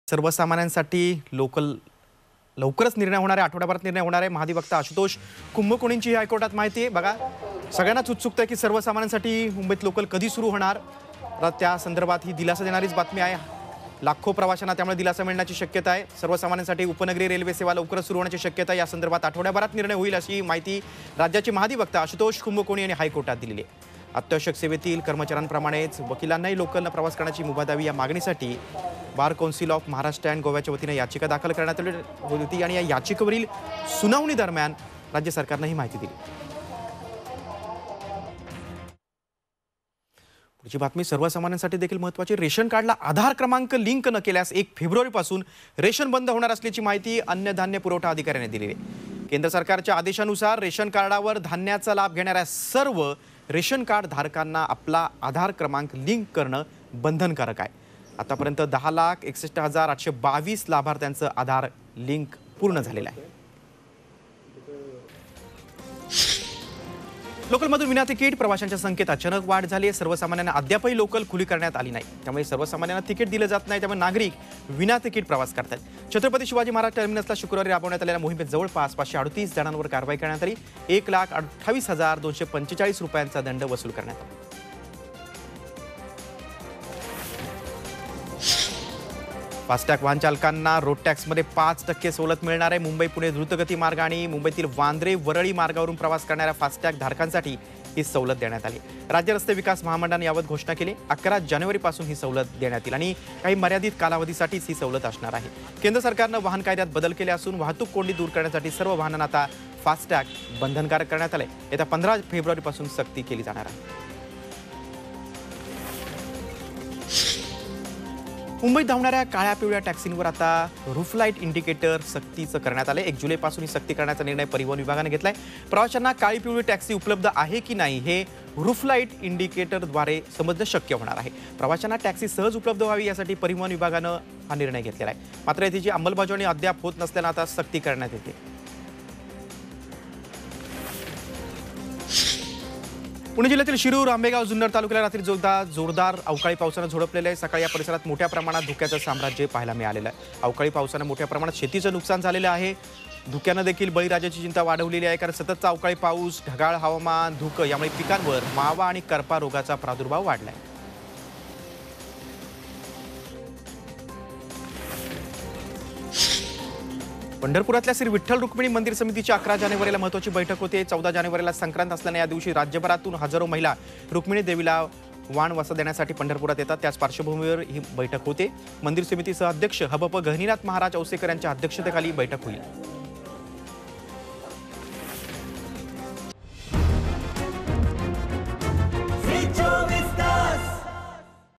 सर्वसामान्यांसाठी लोकल लवकरच...
Maharashtra Government Decision To Start Railway Local Train For Common People Soon